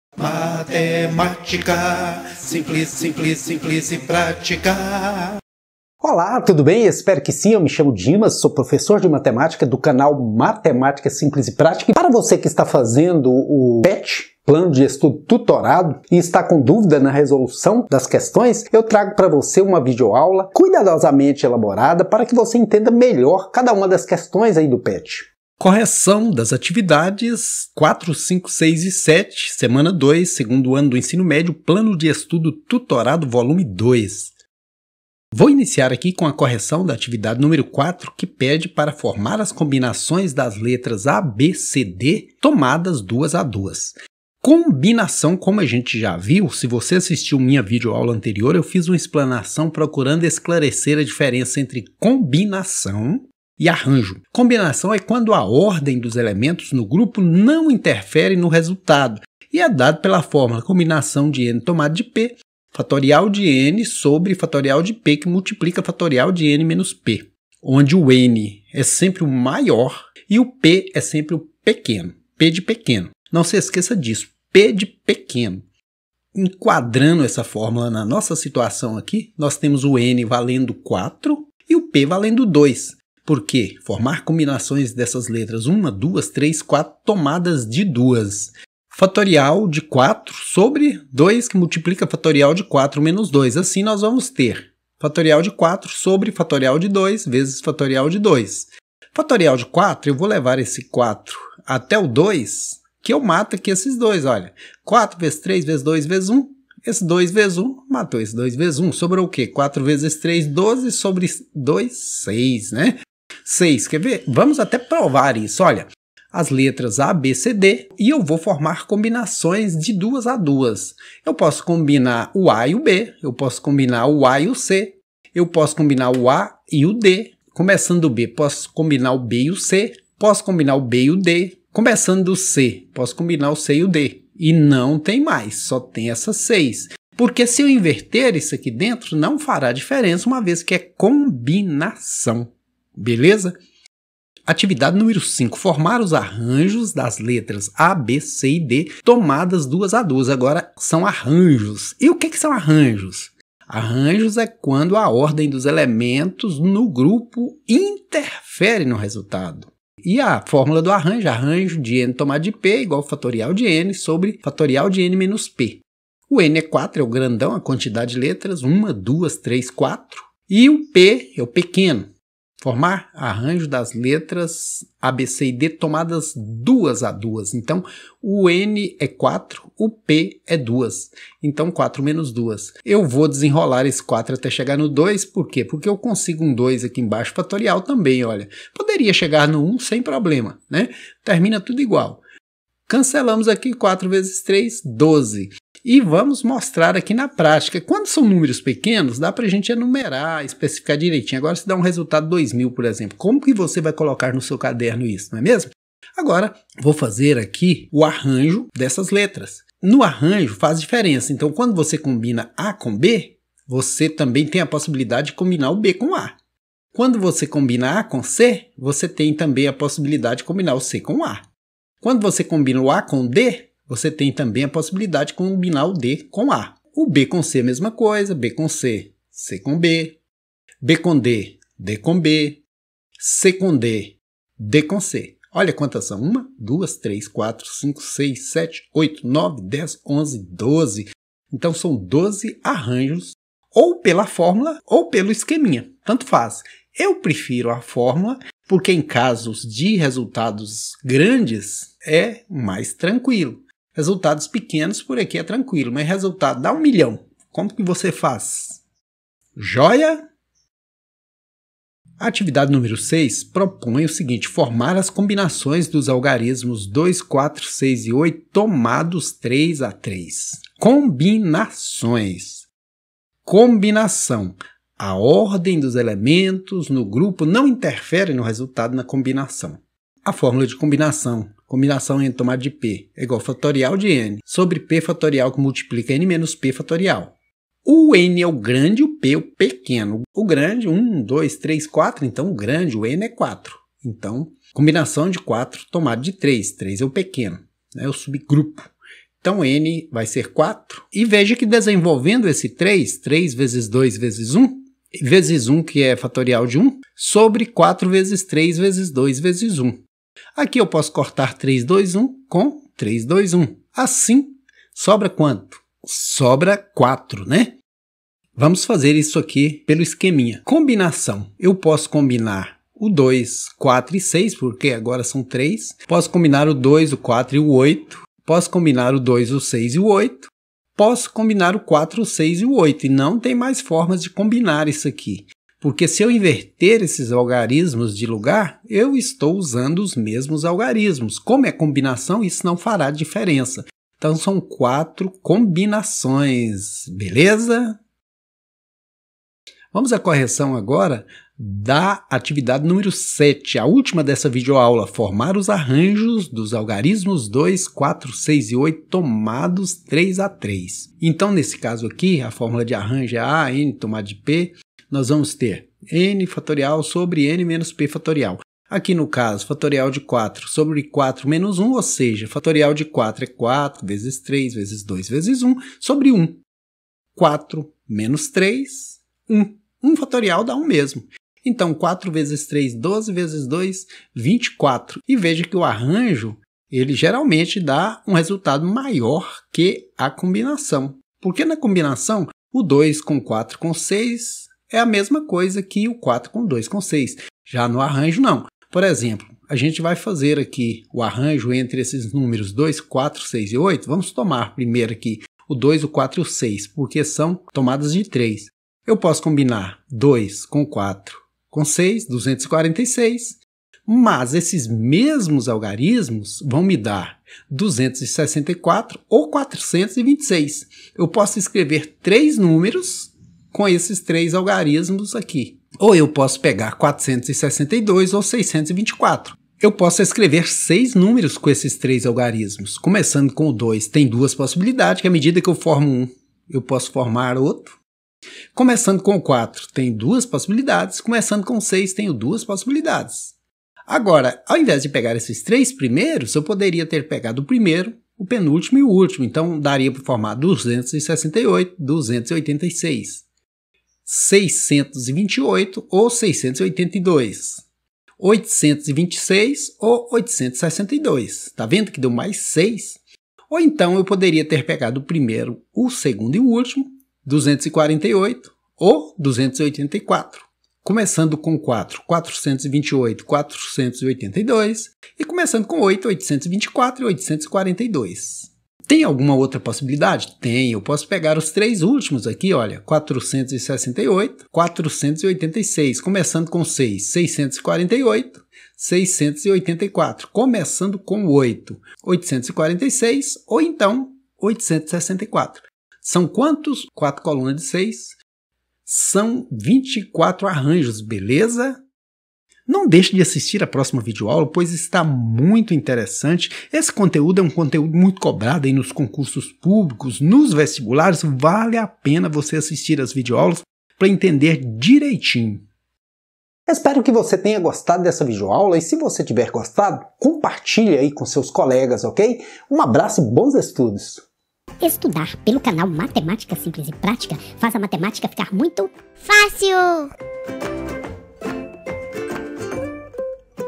Correção das AtividadeS 04, 05, 06 E 07 , Semana 02, PET Vol. 2, 2º Ano Ens. Médio
Nessa vídeo aula eu faço a Correção detalhada, com explicação clara, das Atividades:4 , 5 , 6 e 7 da semana 02, PET(Plano de Estudo Tutorado) Volume 02, do 2º Ano do Ensino Médio.
4- Formar as combinações das letras a , b , c , d tomadas duas a duas.
5 - Formar os arranjos das letras a , b , c , d  tomadas duas a duas.
6-Formar as combinações dos algarismos 2, 4, 6 e 8 tomados três a três
7-Formar os arranjos dos algarismos 2, 4, 6, e 8 tomados três a três.